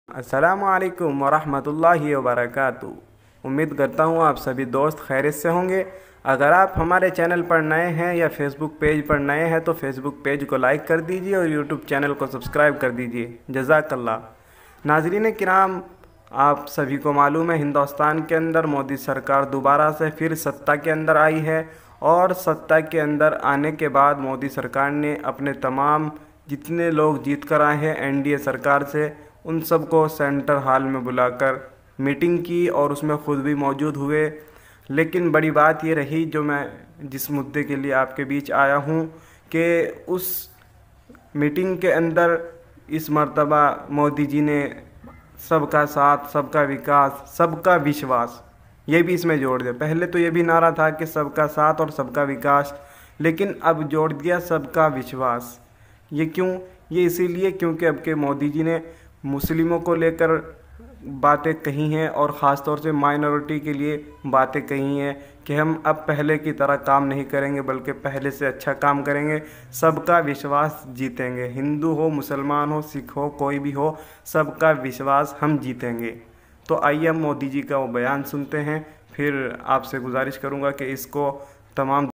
जीत के बाद पहली मीटिंग मुसलमानों के लिए कही बड़ी बात मोदी ने
welcome to Uvåis Qarni Channel

Uvais Qarni is an internet Based islamic world update channel

Voice By_ Muhammad uvais qarni